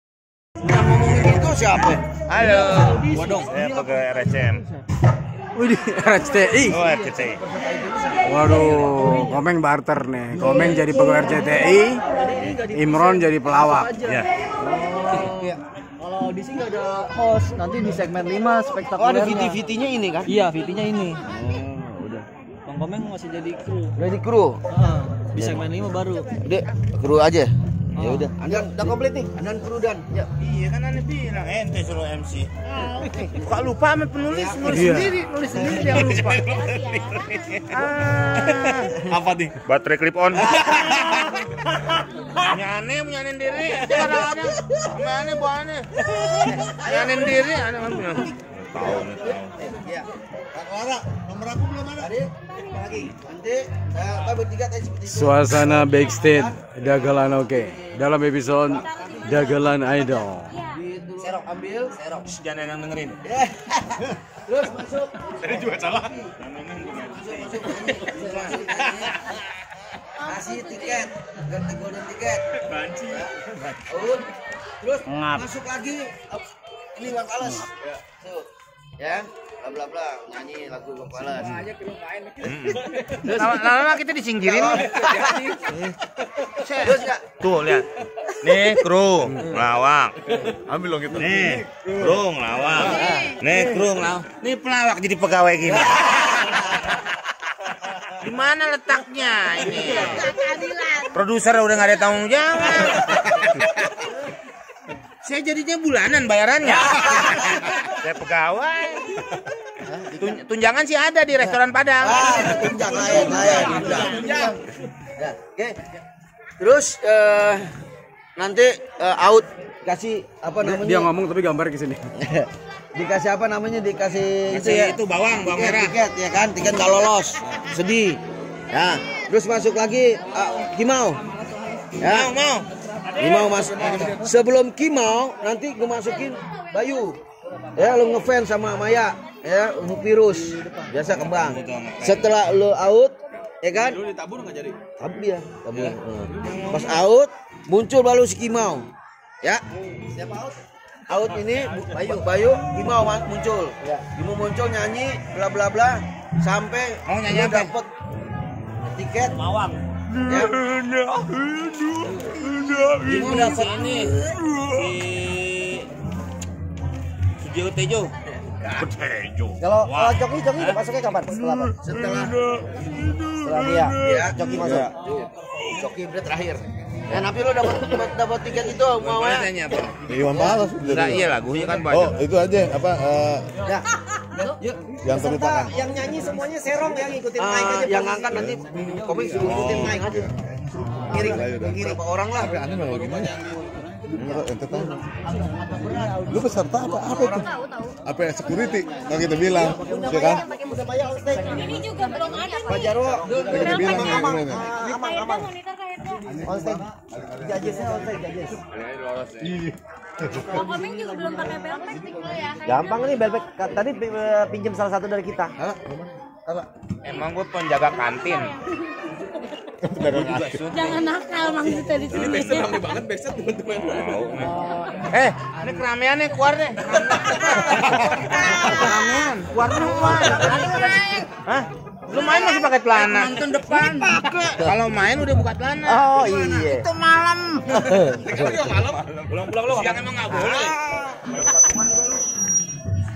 Backstage Dagelan Idol Komeng Jadi Karyawan
Suasana backstage rekaman program Dagelan Oke dng tema Dagelan Idol, Komeng berperan jadi crew RCTI, bbrp crew RCTI ikut melawak